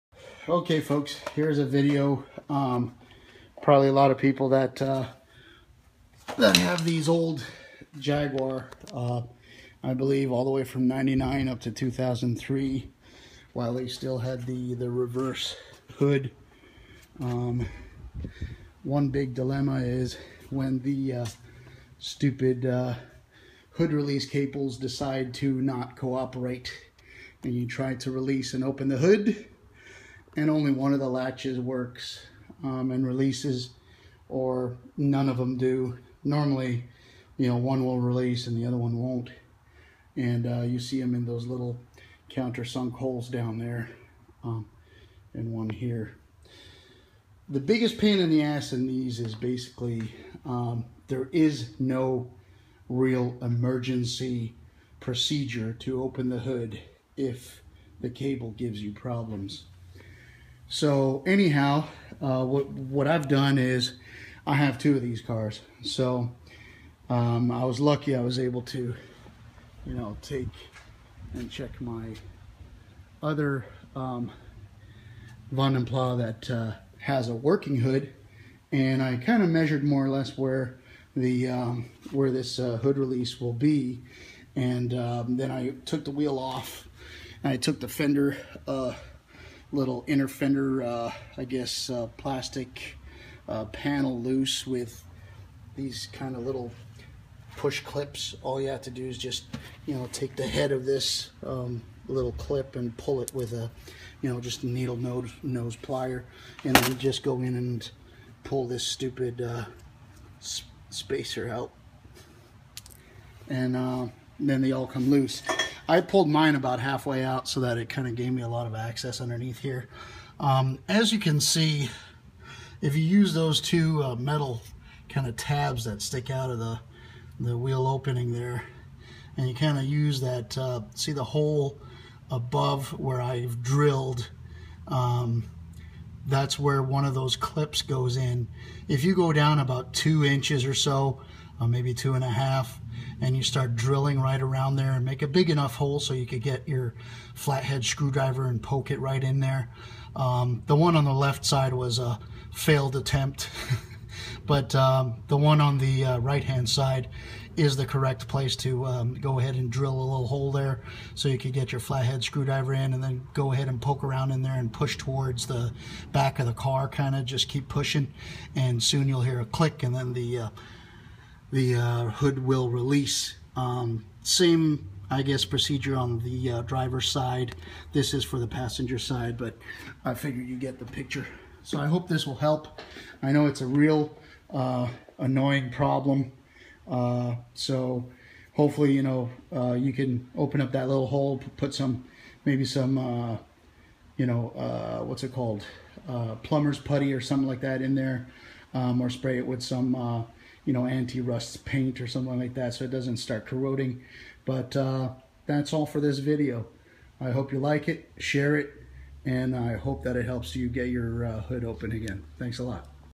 1998-2003 Jaguar XJ8 Vanden Plas Hood Not Opening, How to Fix
This is a quick video to show how to get your hood to open if it will not release once you pull the handle from inside the car. PLEASE DISCONNECT THE BATTERY BEFORE DOING THIS JOB!